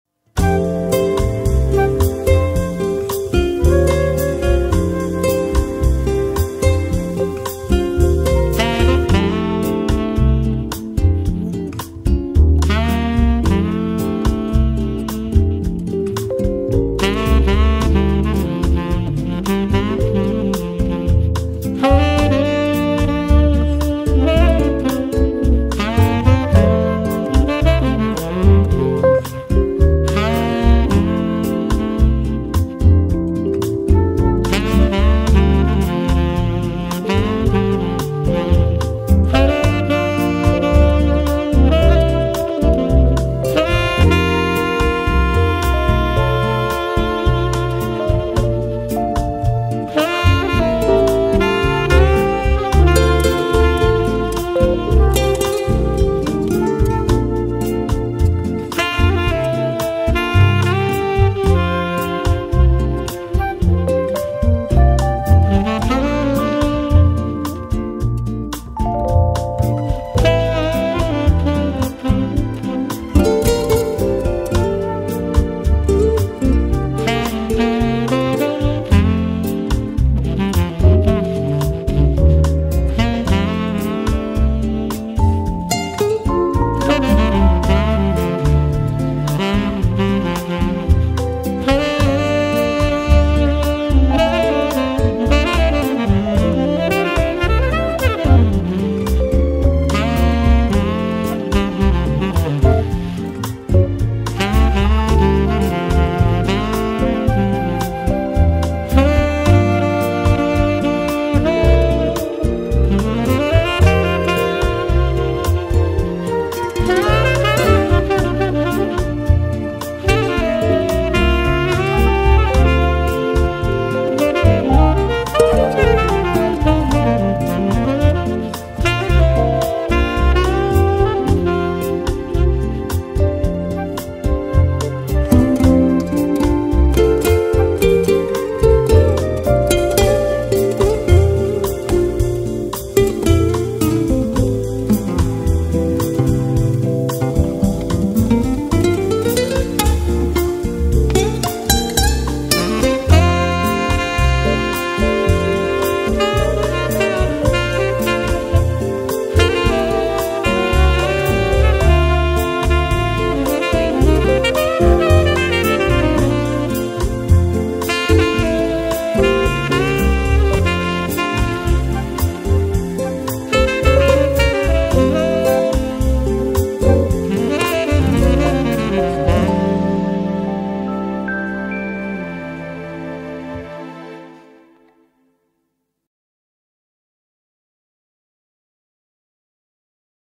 Royal Park Rajaphruek Landscape design 2011 by Suphachai Sinsuebphon
งานออกแบบ landscape สวนกรมอุทยาน งานพืชสวนโลกปี2554 โดย ศุภชัย สินสืบผล